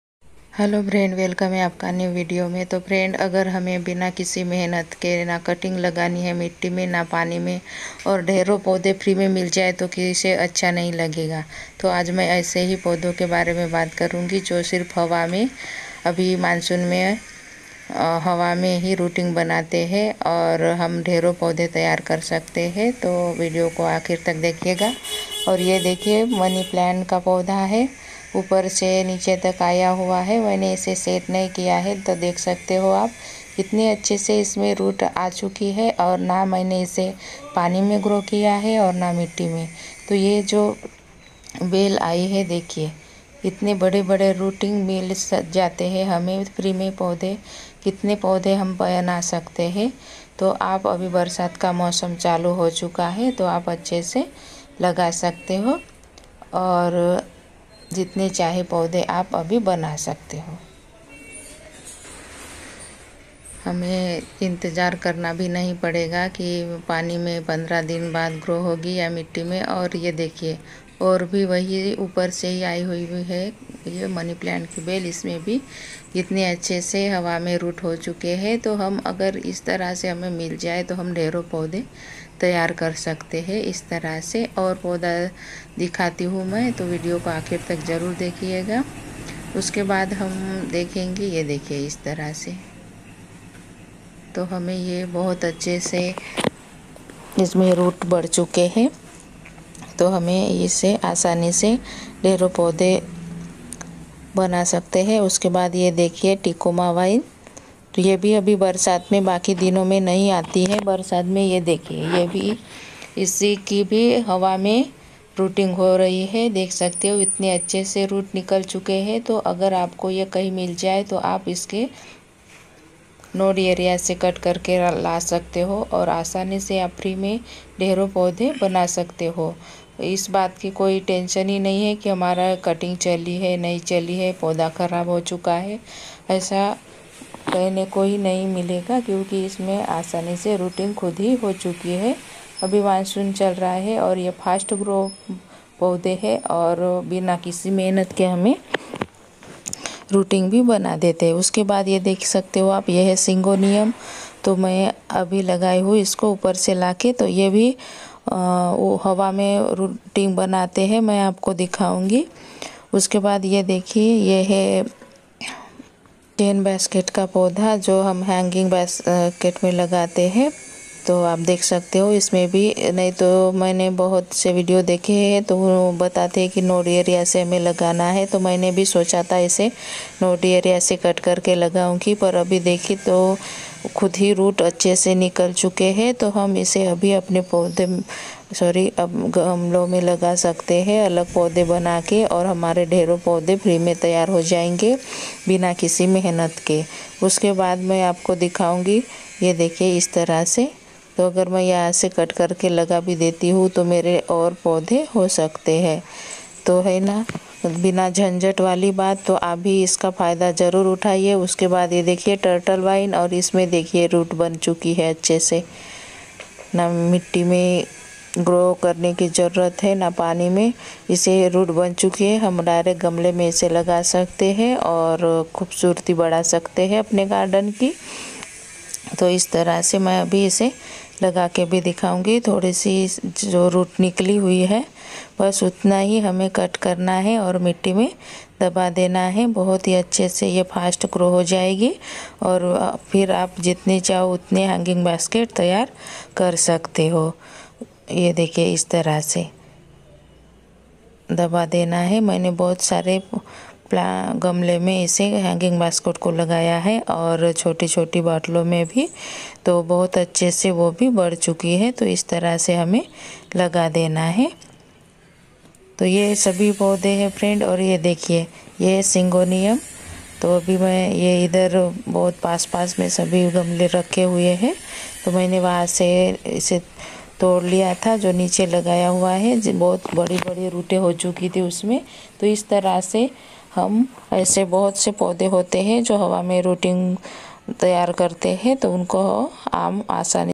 अब् बिना कटिंग grow kre podhe kar sakte ho direct reporting ll ऐसा जादू पौधों मे कभी नही देखा होगा l
bina cutting lgaye tayar milenge dhero podhe abhi bina kisi mehanat ke ll hawa me rut bnane wale podhe ll aise podho ke naam jo mansun me hwa me hi rut banana start karte hain ll air ruttng development plants without grow cutting ll nivansi terice garden ll